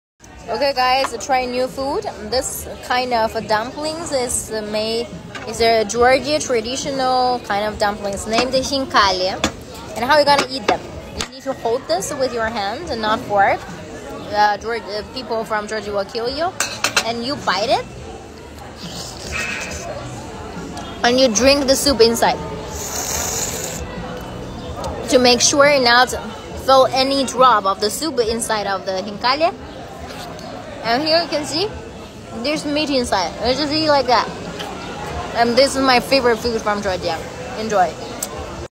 How to eat khinkali - Как есть хинкали
Eating khinkali is also a part of traditional Georgian folklore: the top, where the dough is twisted into a knot, should never be eaten because it serves as a handle to hold the dumpling, and it is usually left on the plate as an indication of how many dumplings have been eaten.